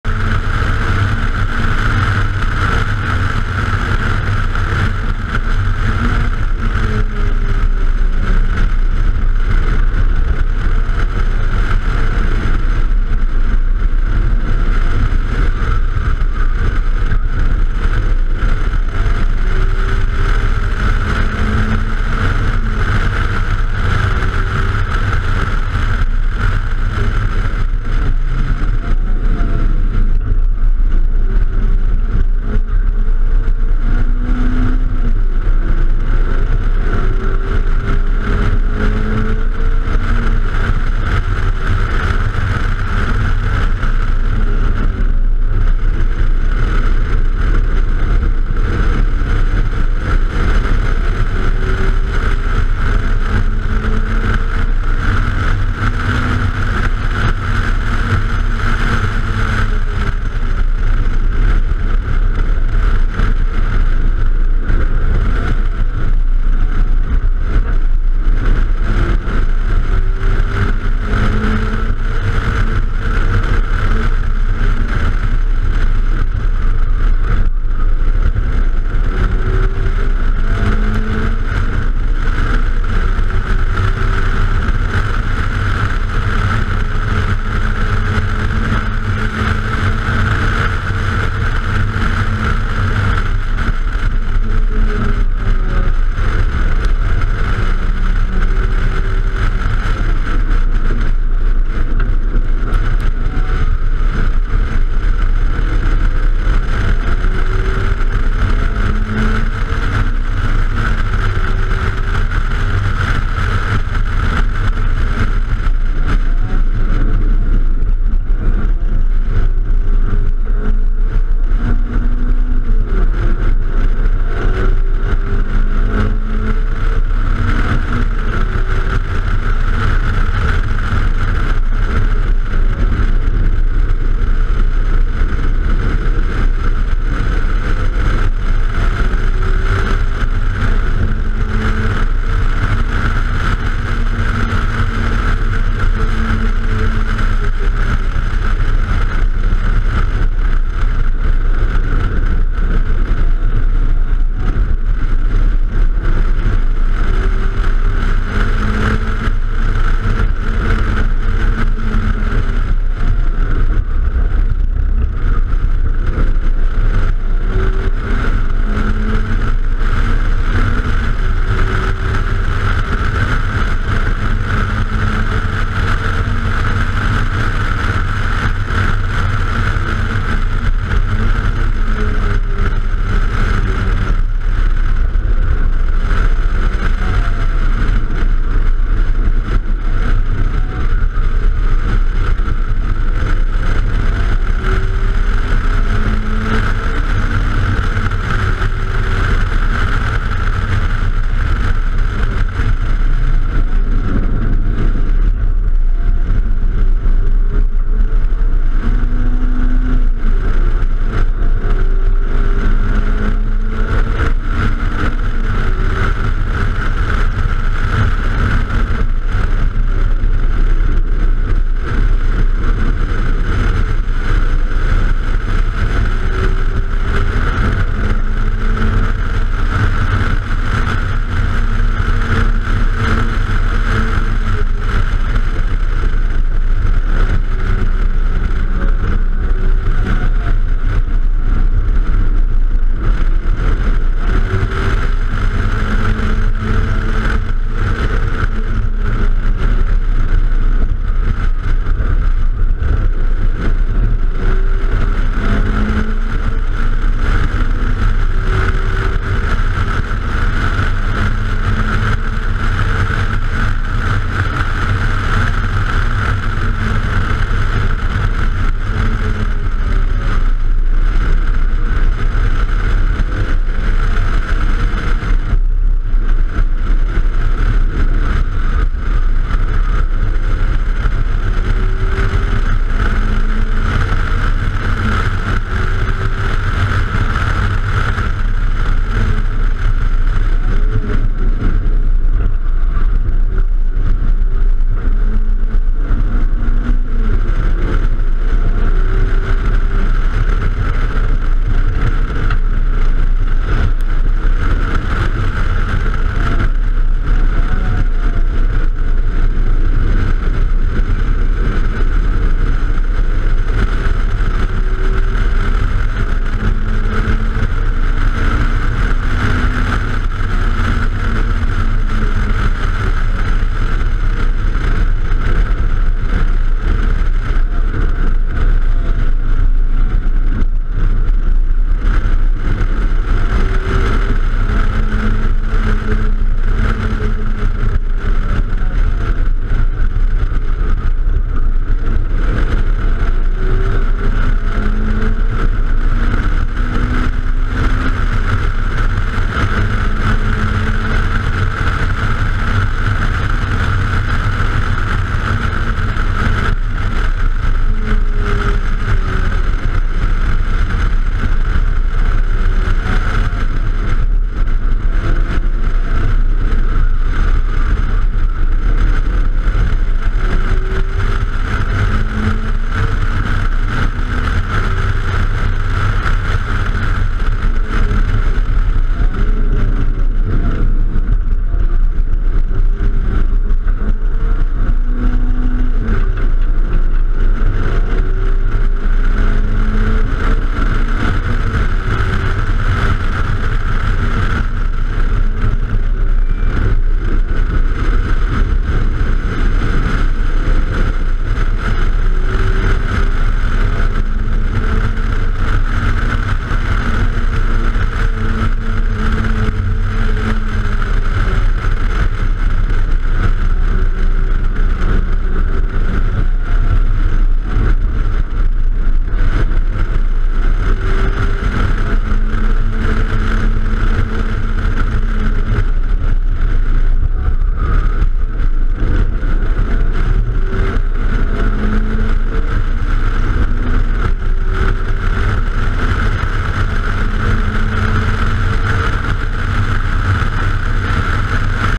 New Hampshire motor speedway road course 5-25-19
track night at NHMS racing the silver hatch. consistent mid 1:27, got a 1:26:9 in there